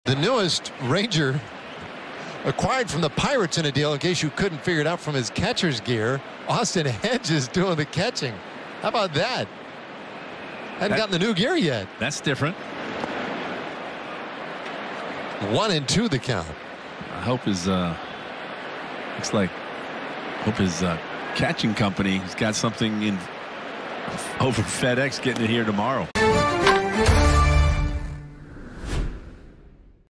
Austin Hedges Wears Pirates Gear While Catching for the Rangers
This is what you get with the trade deadline. Lots of guys getting moved quick and no time to get ready.